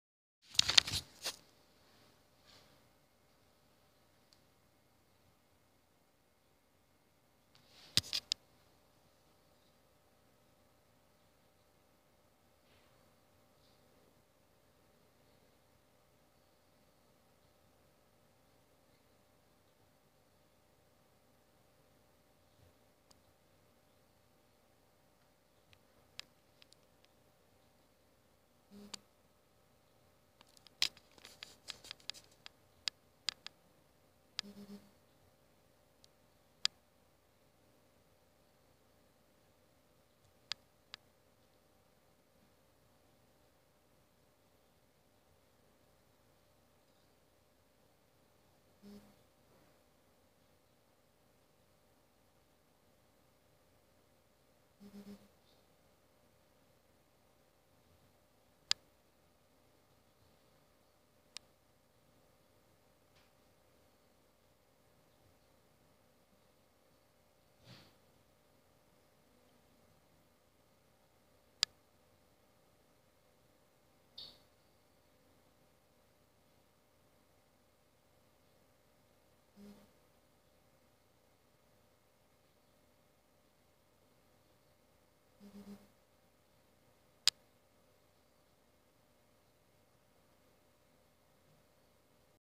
Samsung S 7 Edge Duos
Samsung S 7 Edge Duos startet immer wieder neu .